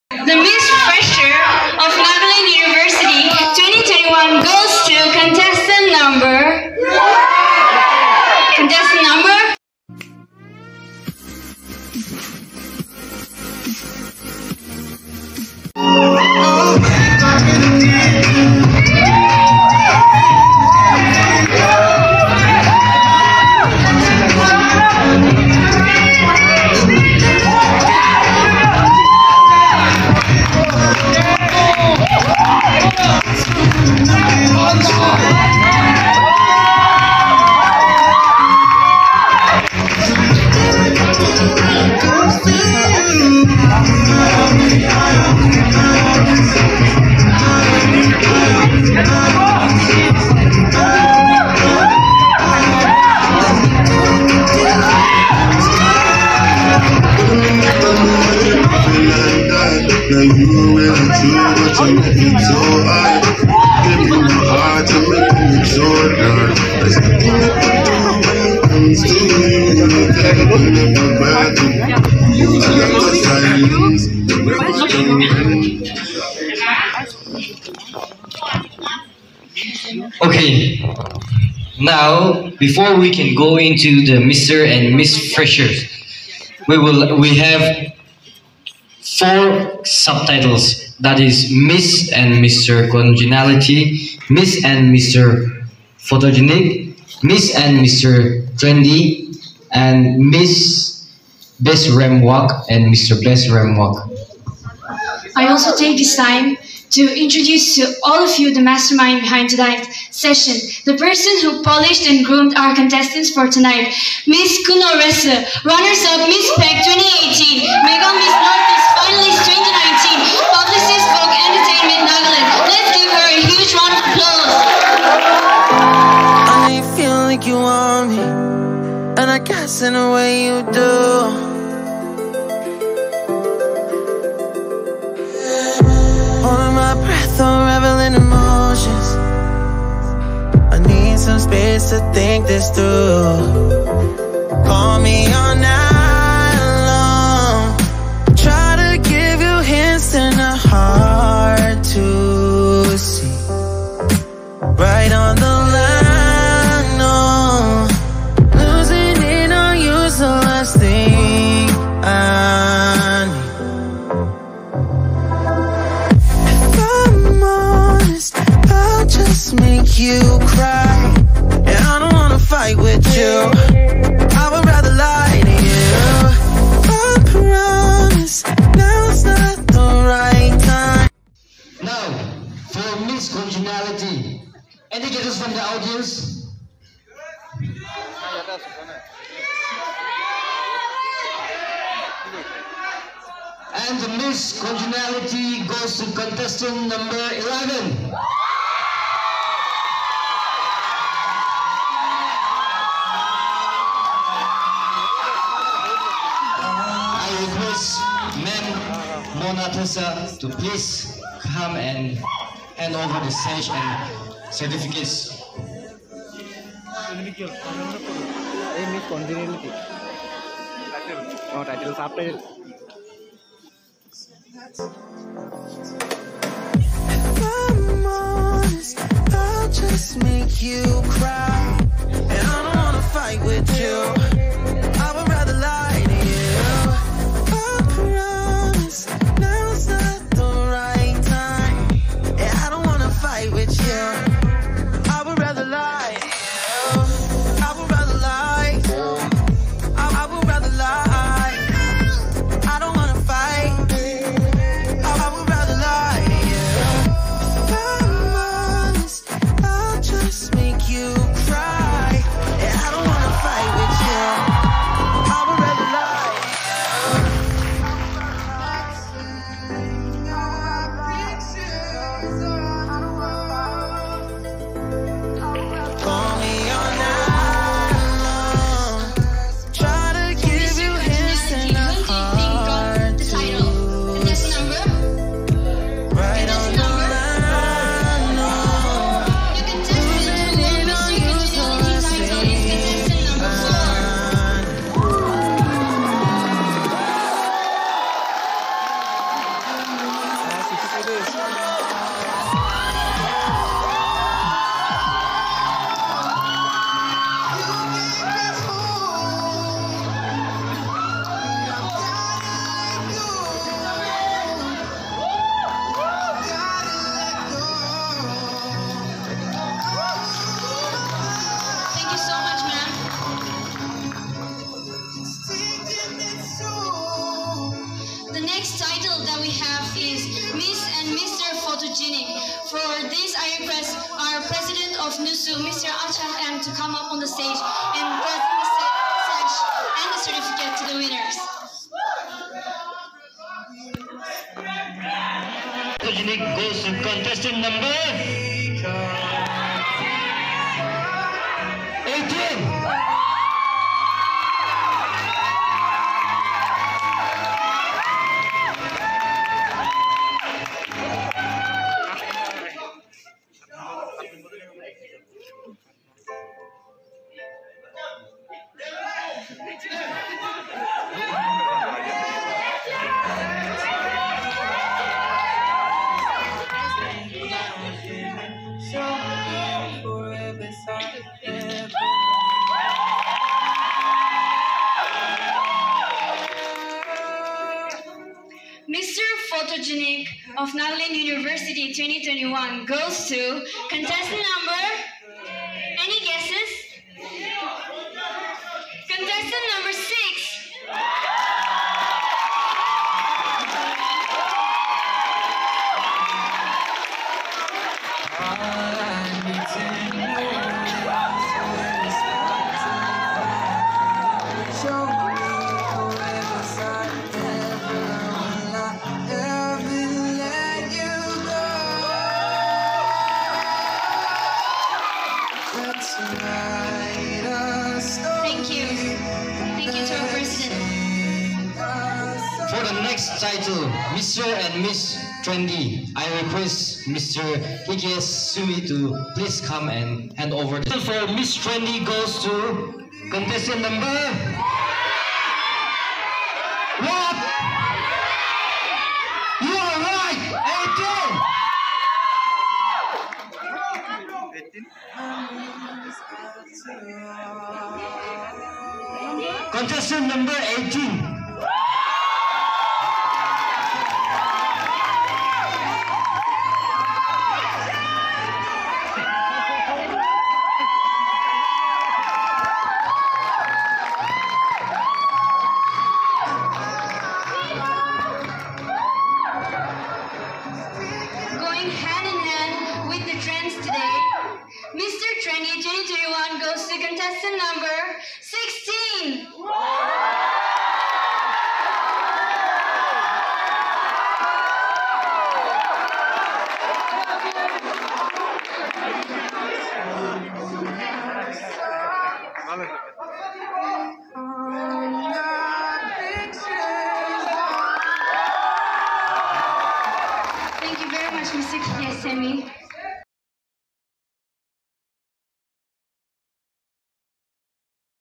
23rd FRESHER'S DAY | CROWING ROUND & FELICITATION | NAGALAND UNIVERSITY, LUMAMI CAMPUS | PART-1
Nagaland University, Lumami Campus celebration its 23rd Fresher's Day on 20th November, 2021 under the initiative of Nagaland University Students' Union.
 The felicitation was done based on
1. MISS CONGENALITY
2. MISS PHOTOGENIC
3. MR & MISS TRENDY
4. MR & MISS FRESHER 2021

Please watch the video and enjoy the moments!

For more updates, please follow me on, Facebook Instagram Twitter